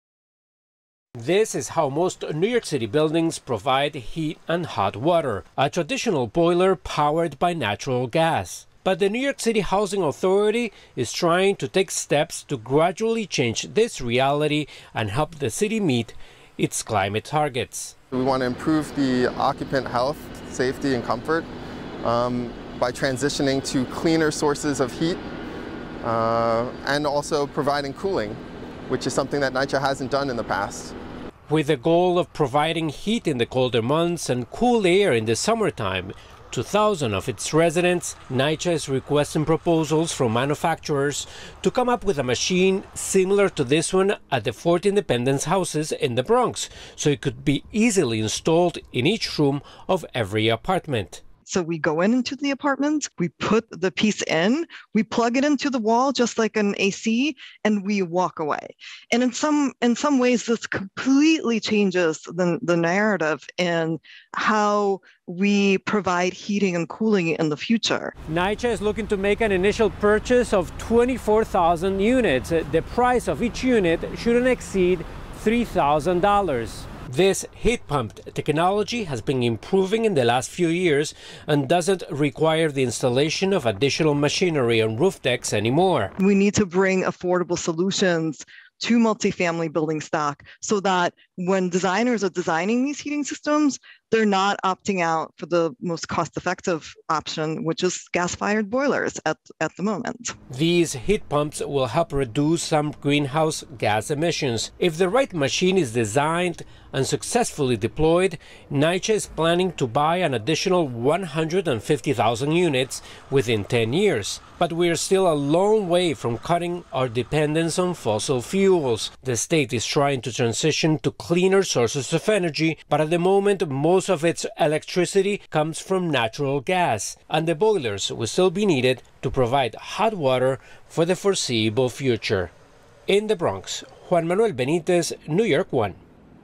NY1: NYCHA looks to clean technology for heating and cooling
Gas-fueled boilers are the traditional way New York City buildings provide heat and hot water.

But the New York City Housing Authority is trying to take steps to gradually change this reality and help the city meet its climate targets.

Most of the electricity in New York State is still produced by natural gas
“We want to improve the occupants' health, safety and comfort by transitioning to cleaner sources of heat and also providing cooling, which is something NYCHA hasn’t done in the past,” Jordan Bonomo, program manager at NYCHA's capital projects energy and sustainability, said.

With the goal of providing heat in the colder months and cool air in the summertime to thousands of its residents, NYCHA is requesting proposals from manufacturers to come up with a machine similar to one already in use at the Fort Independence Houses in the Bronx, so it could be easily installed in each room of every apartment.

“So we go into the apartments, we put the piece in, we plug it into the wall just like an AC and we walk away," Vlada Kenniff, NYCHA's vice president of energy and sustainability, said. "And in some ways that completely changes the narrative and how we provide heating and cooling in the future."

NYCHA is looking to make an initial purchase of 24,000 units. The price of each unit shouldn’t exceed $3,000.

Heat pump technology has improved over the last few years and doesn’t require the installation of additional machinery on roof decks anymore.

“We need to bring affordable solutions to multi-family building stock so when designers are designing these heating systems, they are not opting out for the most cost-effective option, which is gas-fired boilers at the moment,” Kenniff said.

Heat pumps will help reduce some greenhouse gas emissions.

If the right machine is designed and successfully deployed, NYCHA is planning to buy an additional 150,000 units within ten years.

But we are still a long way from cutting our dependence on fossil fuels.  The state is trying to transition to cleaner sources of energy, but at the moment most of its electricity comes from natural gas. And the boilers will still be needed to provide hot water for the foreseeable future.

by NY1 News